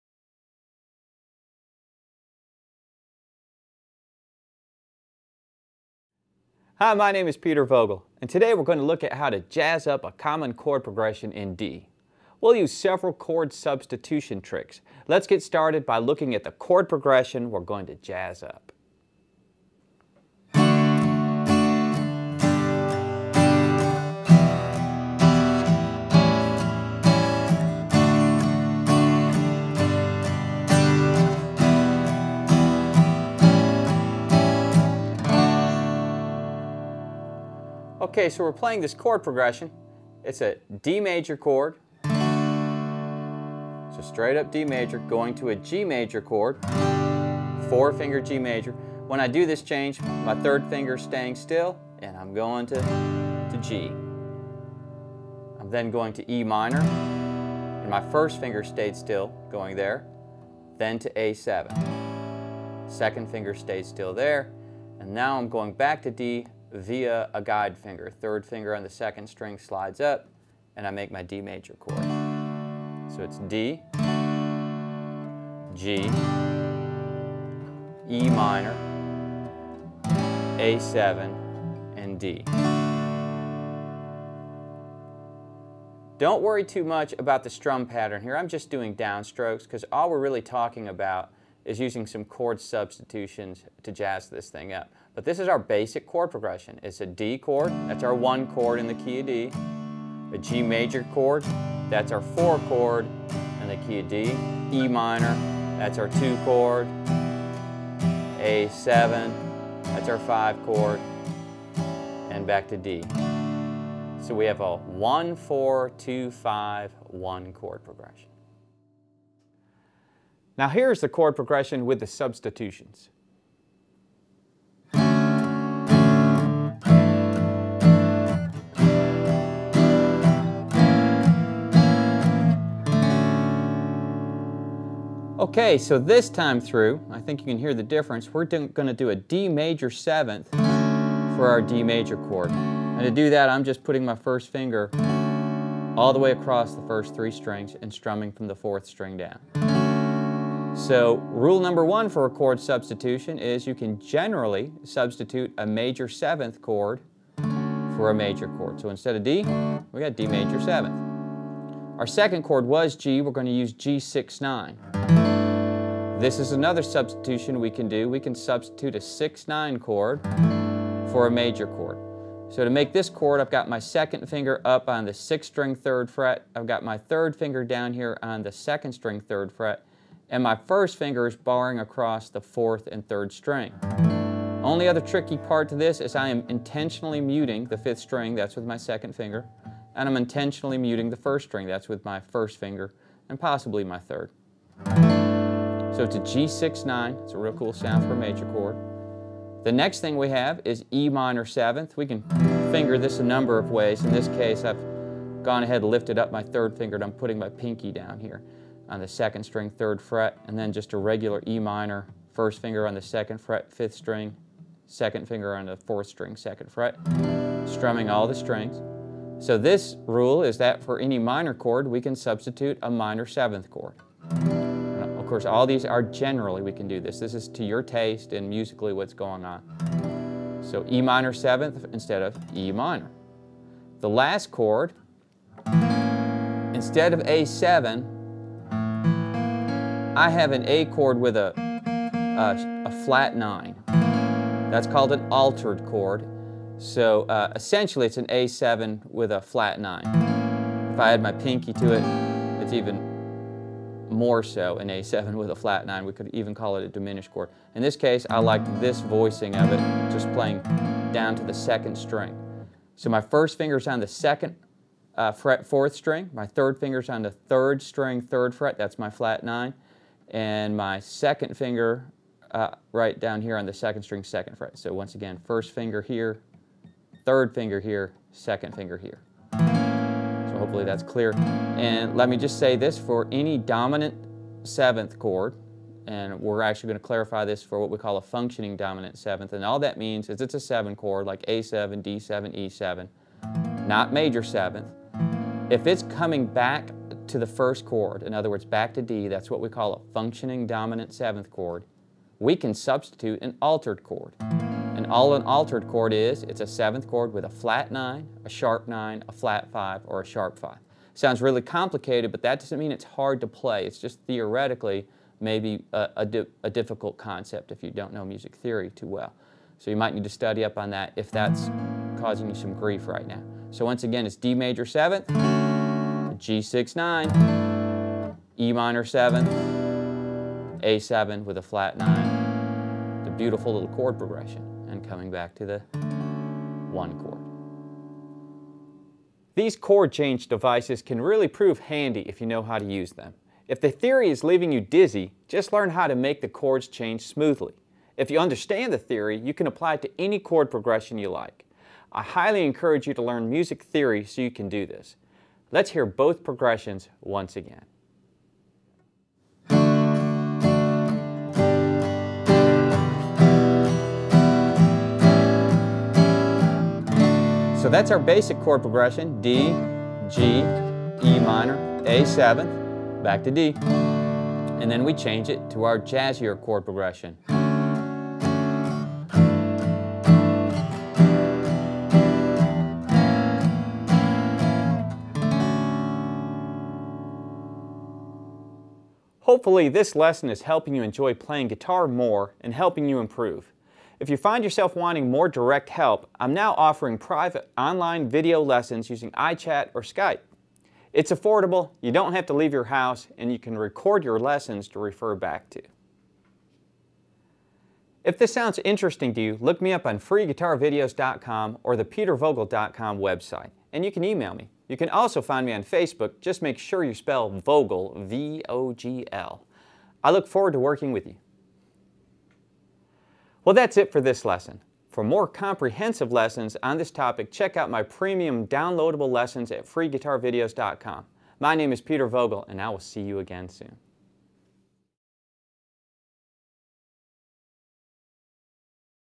Beginning to Jazz Up a Chord Progression
Beginning to Jazz Up a Chord Progression is a free video lesson by Peter Vogl that will teach you about using chord substitutions. We will start by learning a simple progression of D, G, Em, and A7. We will then learn how to play the progression with a substitution for each of those chords. Using chords subs allows you to avoid the monotony of long repetitive chord progression. These can be especially useful when you're playing along with a singer or playing an extended jam. To learn more about this, check out Peter's premium lesson Jazz Up This Chord Progression.